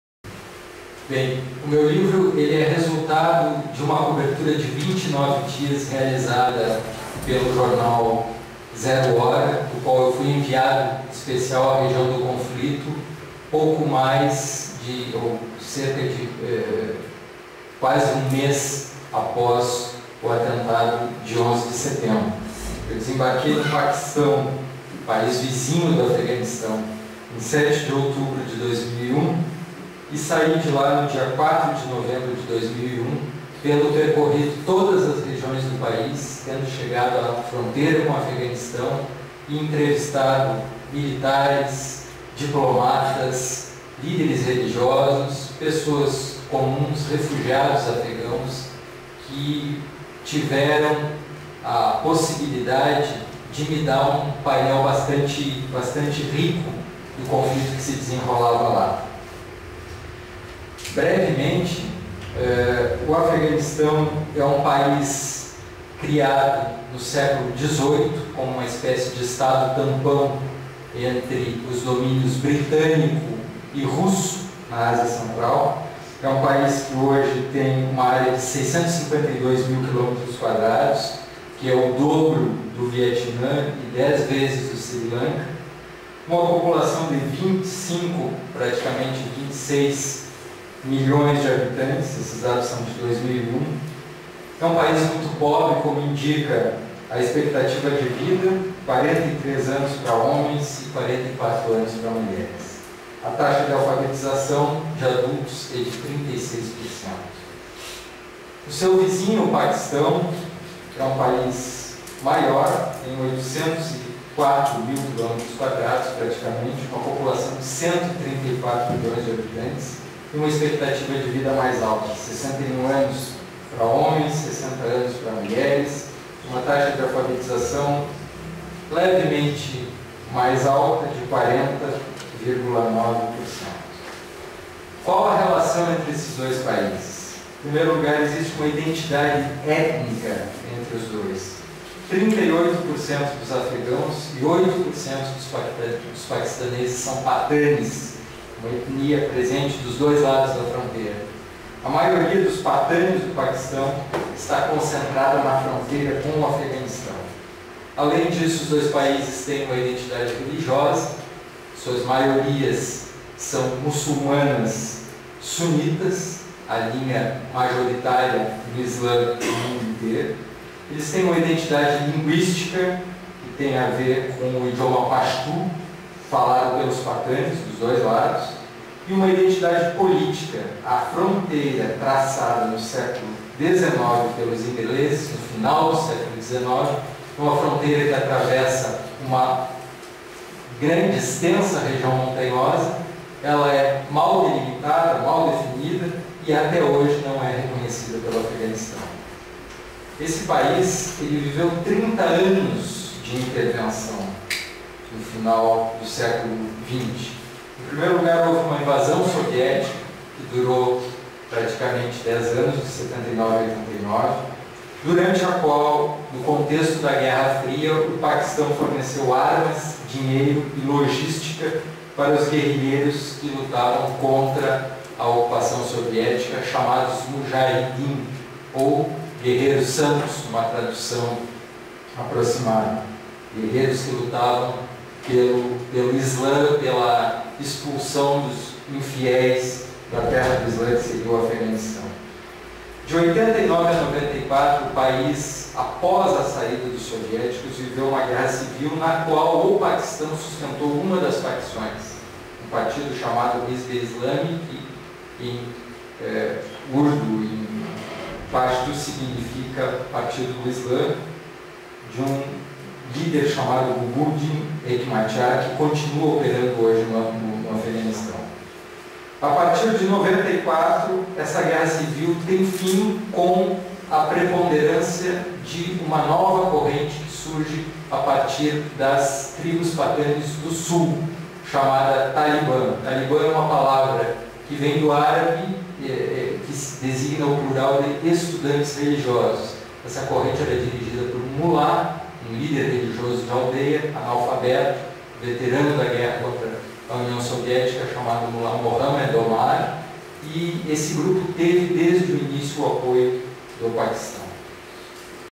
Luiz Antonio Araújo fala sobre sua experiência no Afeganistão
Repórter da Zero Hora participa do Cultura na SEDUFSM sobre "o papel da imprensa nas guerras do Oriente Médio".